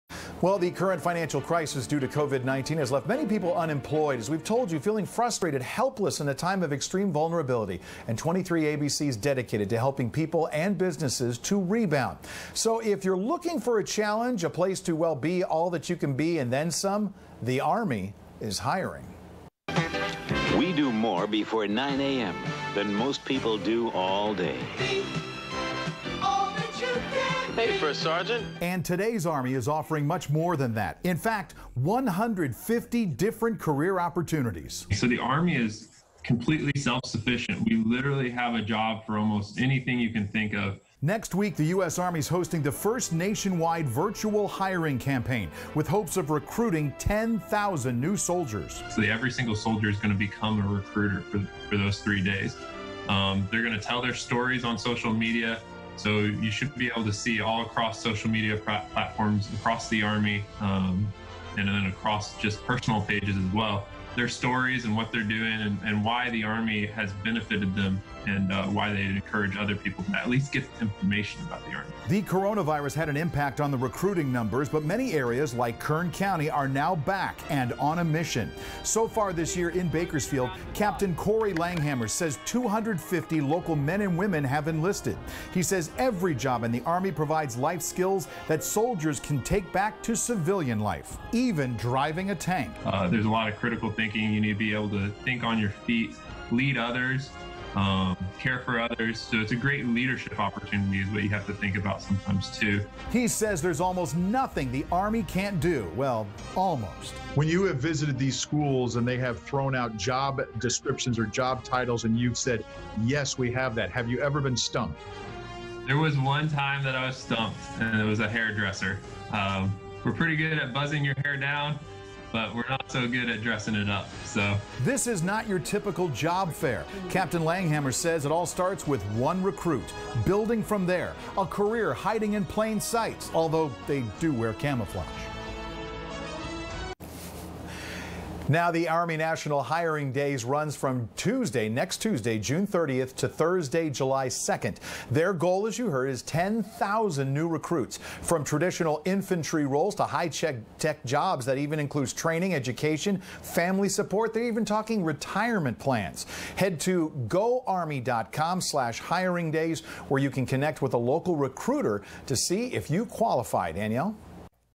The Rebound Kern County: Army National Hiring Days, June 30 - July 2
The current financial crisis due to COVID-19 has left many people unemployed, feeling frustrated and helpless in a time of extreme vulnerability. So 23ABC is dedicated to helping individuals and businesses to "rebound." And if you're looking for a challenge, a place to "be all that you can be and then some - the army is hiring.